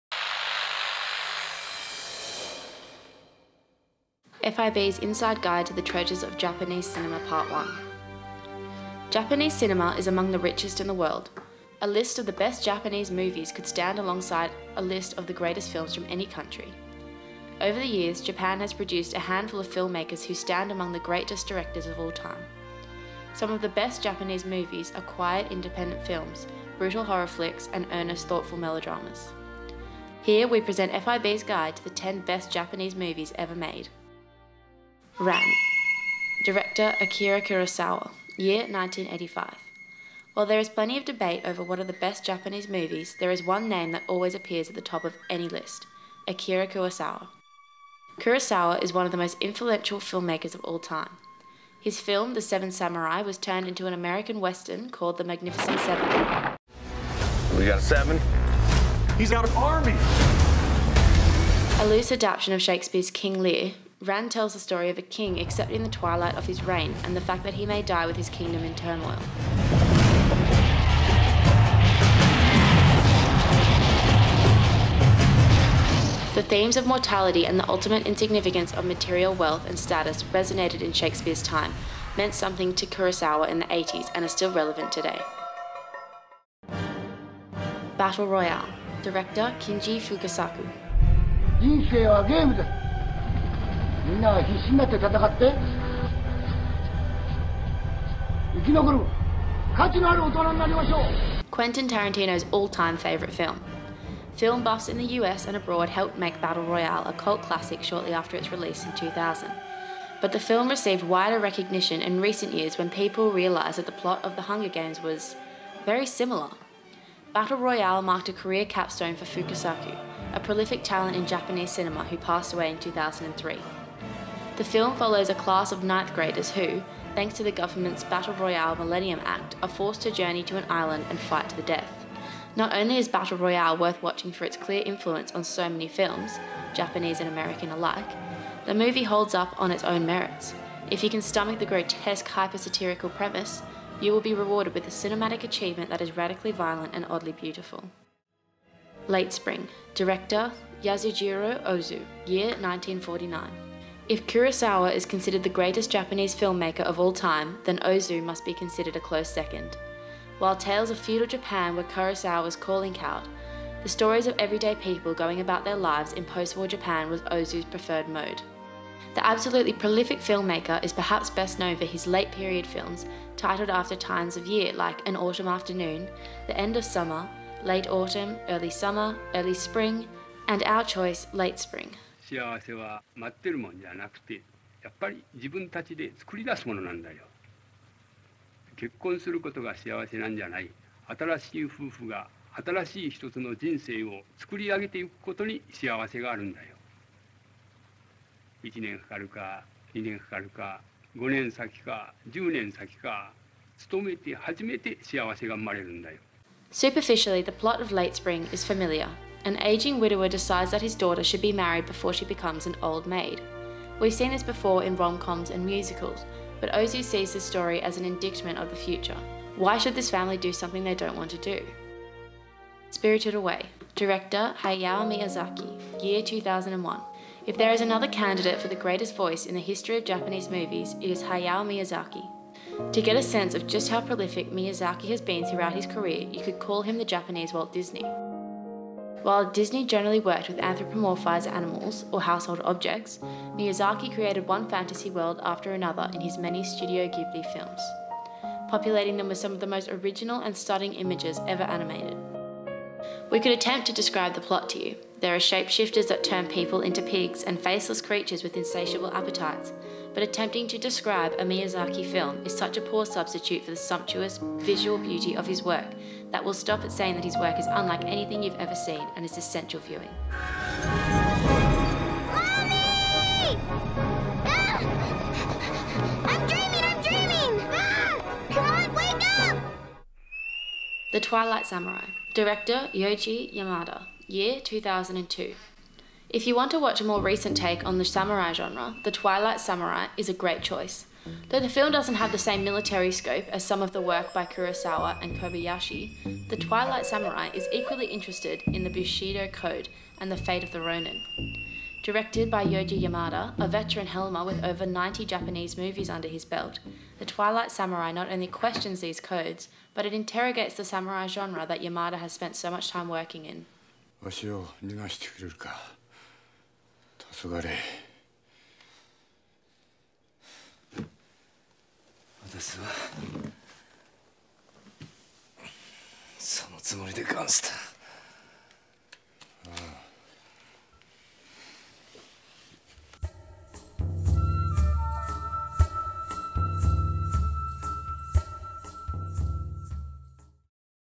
FIB JAPANESE CINEMA TREASURES PART 1
Japanese cinema is among the richest in the world. A list of the best Japanese movies
could stand alongside of a list of the greatest films from any country. Over the years,
Japan has produced a handful of filmmakers who stand among the greatest directors of
all time.
Some of the best Japanese movies are quiet independent films, brutal horror flicks, and
earnest, thoughtful melodramas.

To make sure you get all the news on Japanese cinema treasures and more sign up for FIB's
▶VIMEO: Fashion Industry Broadcast
▶EDITOR: Beth Toh